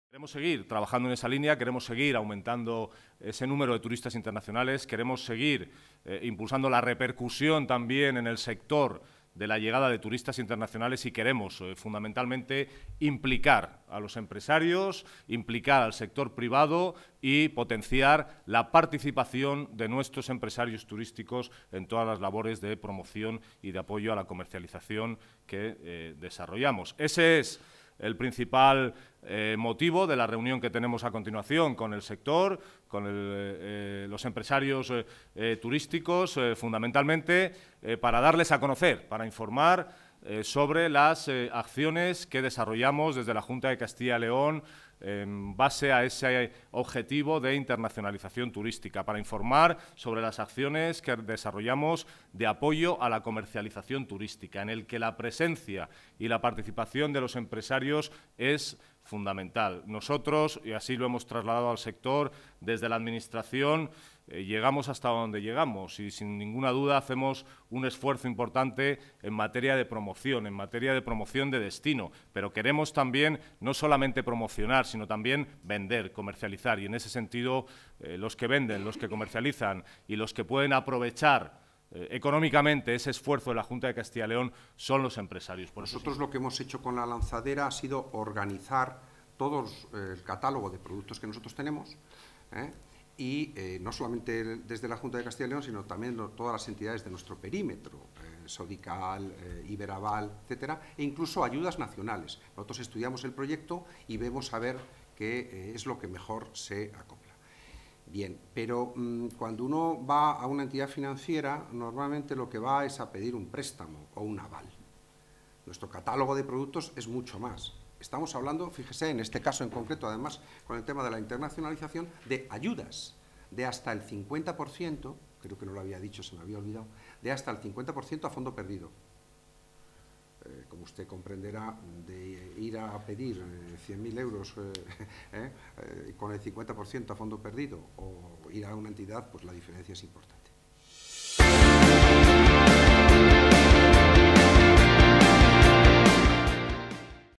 La Junta quiere impulsar la internacionalización de las empresas turísticas segovianas
La Junta de Castilla y León quiere impulsar la internacionalización de las empresas turísticas de la región en general y de Segovia en particular. Con ese fin, los directores de Turismo, Javier Ramírez y de la Agencia de Innovación, Financiación e Internacionalización, José María Ribot, junto al delegado territorial, Javier López-Escobar, presentaron este miércoles en Segovia, ante un nutrido grupo de empresarios turísticos, sus Líneas de Apoyo a la Internacionalización Turística.